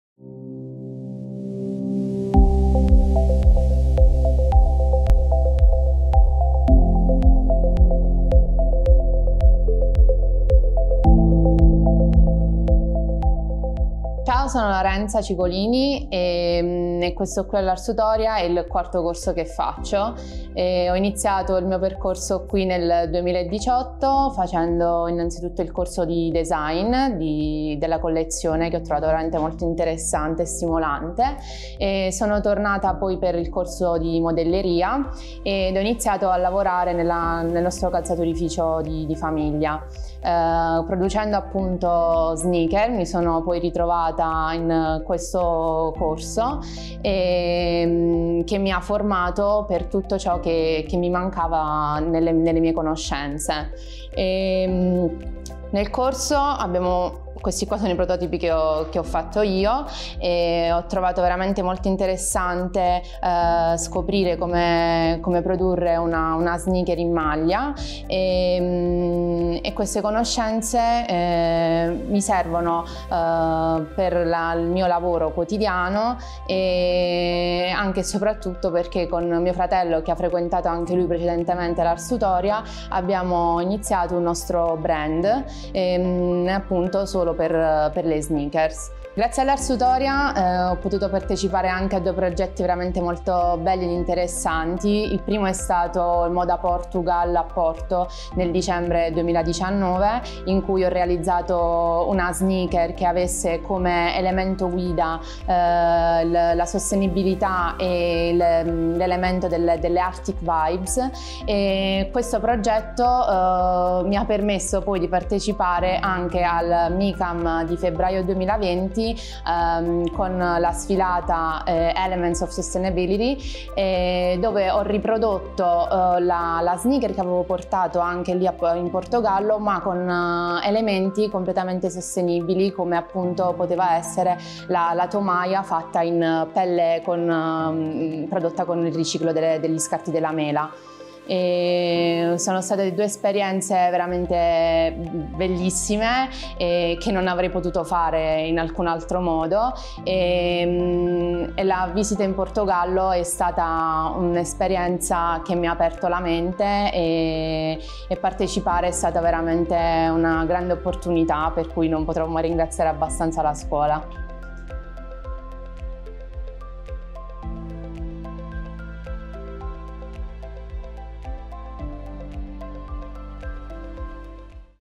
Meet our Talents: Lorenza Cicolini
Lorenza Cicolini racconta la sua esperienza ad Arsutoria School, dove ha seguito 4 corsi di specializzazione nell'ambito delle calzature.